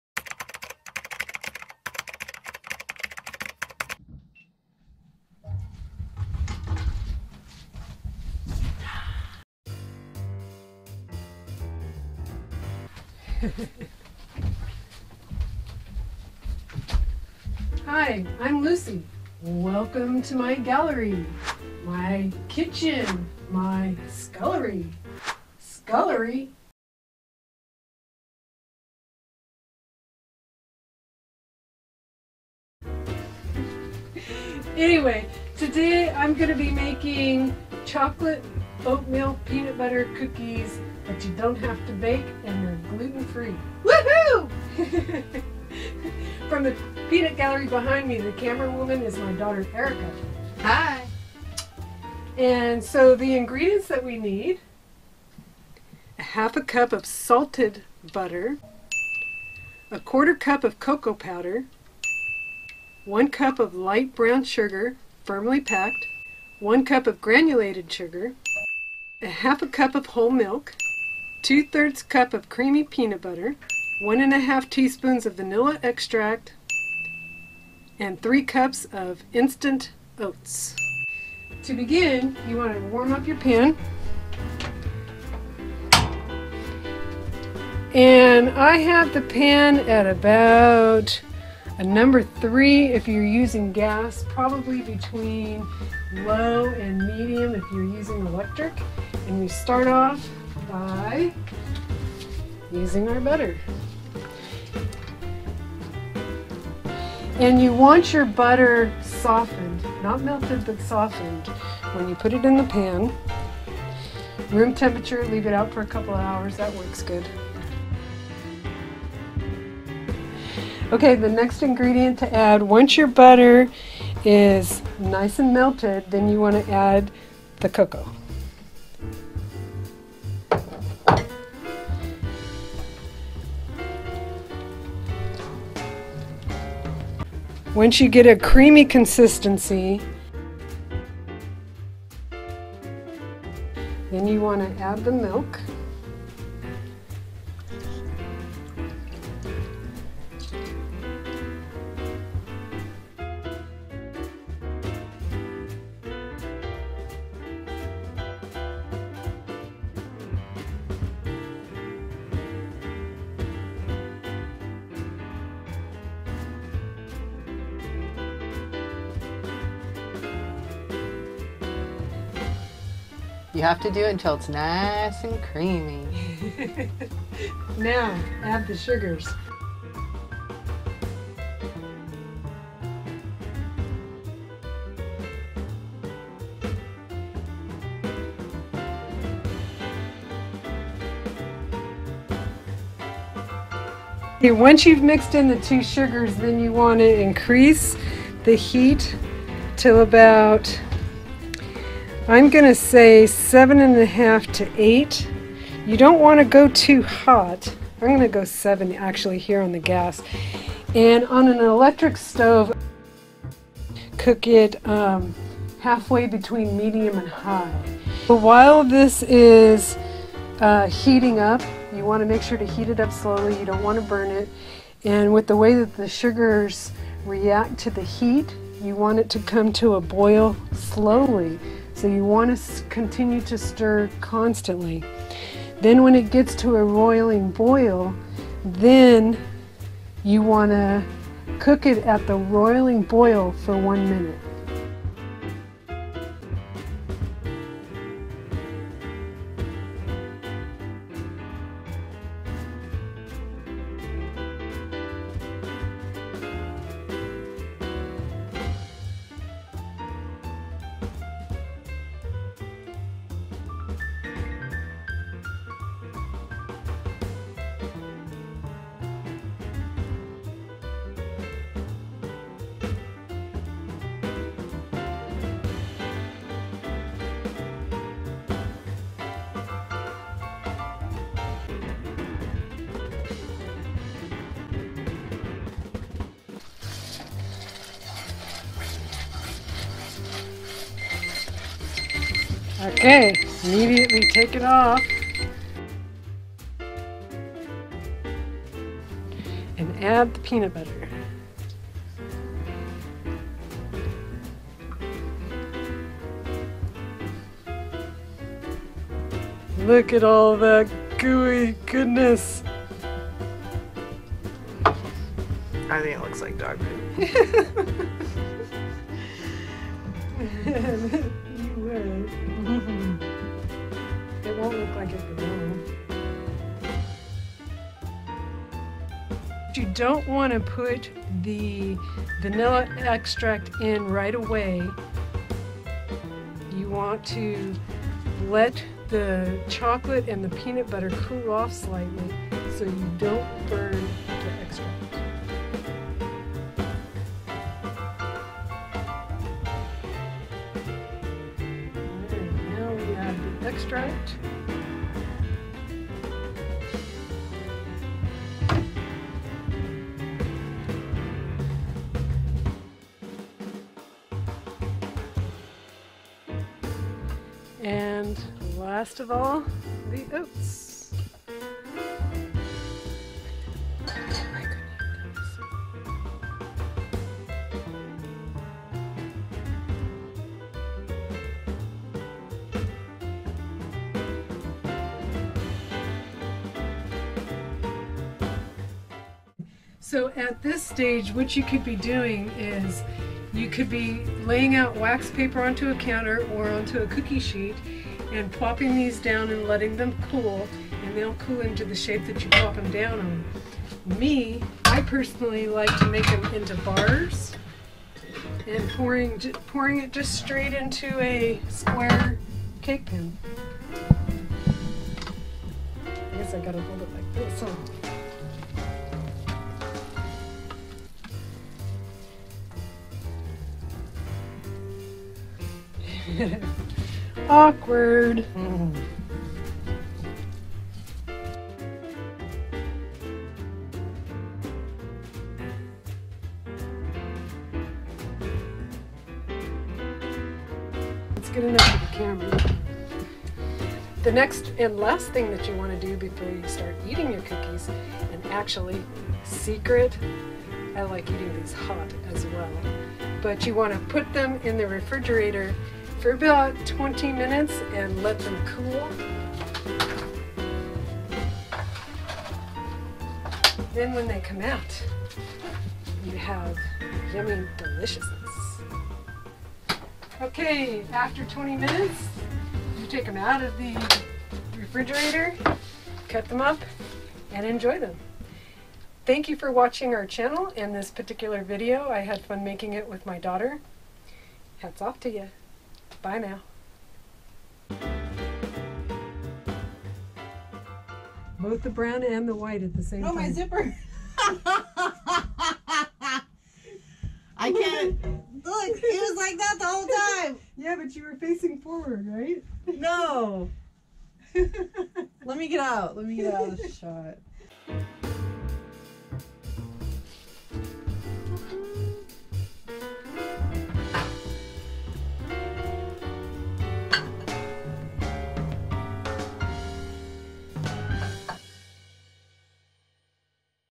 No Bake Chocolate Peanut Butter Oatmeal Cookies Gluten Free - RV Life
Come join us as we show you how to make cook-top cookies that are gluten free.  I had fun making these cookies with my daughter in our RV.

Are regular Quaker Oats gluten free?
Here's the statement from Quaker Oats regarding this problem: "Yes, oats are naturally gluten-free. However, during farming, transportation and storage, gluten-containing grains like wheat, rye, barley and spelt may be unintentionally introduced." ... However, some people will have reactions even with pure oats.May 8, 2017

Camera used: GoPro Hero Black 5 & 6
software: Cyberlink Power Director 17

We really appreciate you coming by and watching our video.

Hat's off to Ya! Bye Now.

0:0 introduction
0:55 ingredients